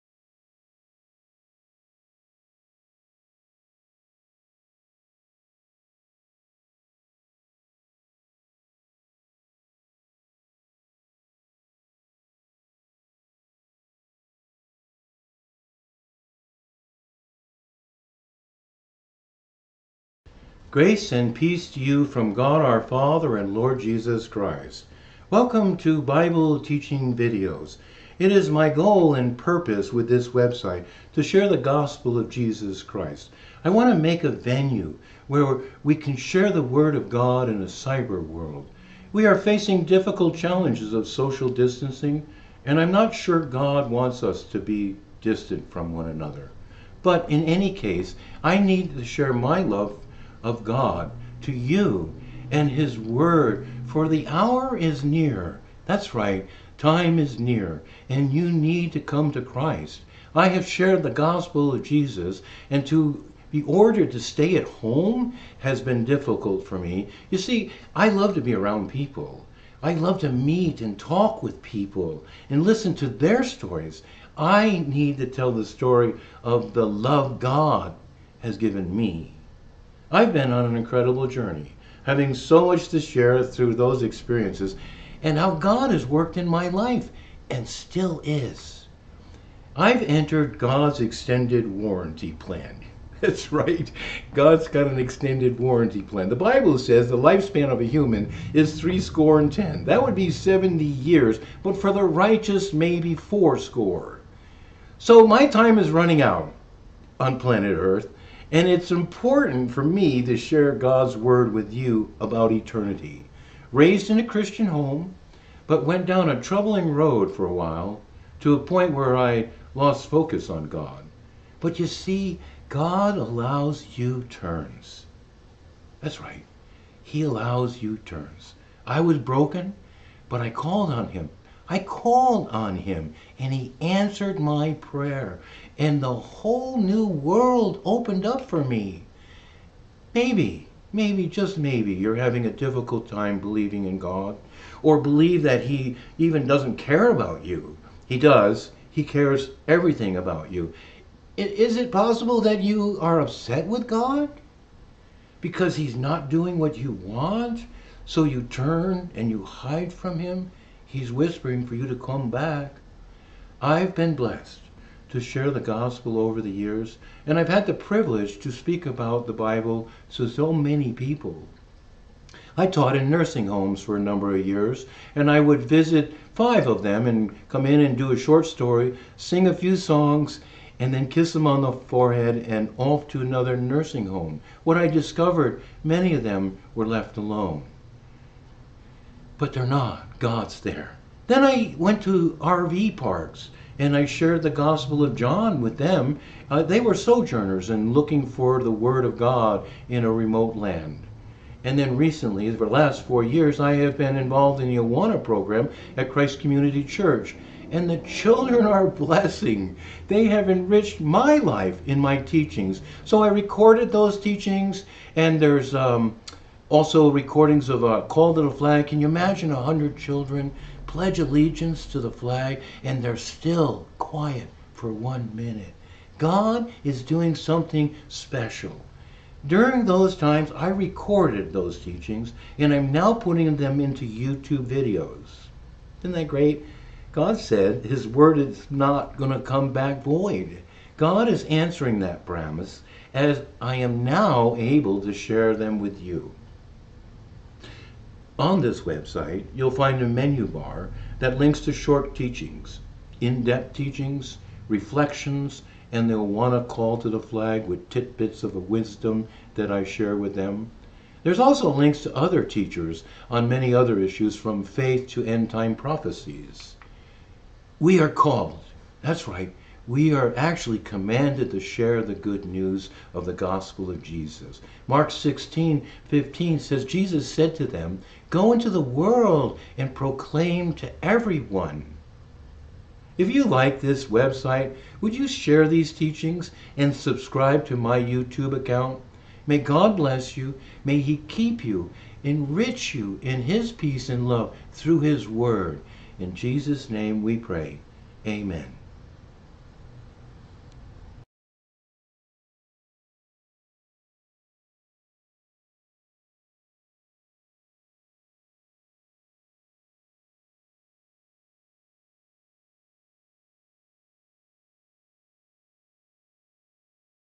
Bible Teaching Videos - Dennis Dwyer M.Div.
This video is an introduction to the website Bible Teaching Videos.com and a tool to be used to share the Gospel of Jesus Christ. There are short teachings, children's teachings, and in depth teachings, reflections and links to other teachers who share about faith and teachings of prophecies of things to come. Dennis Dwyer is a graduate of Phoenix Seminary with a Masters in Divinity and loves to share the Word of God. So, come join us on a digital journey through the Bible. God bless you!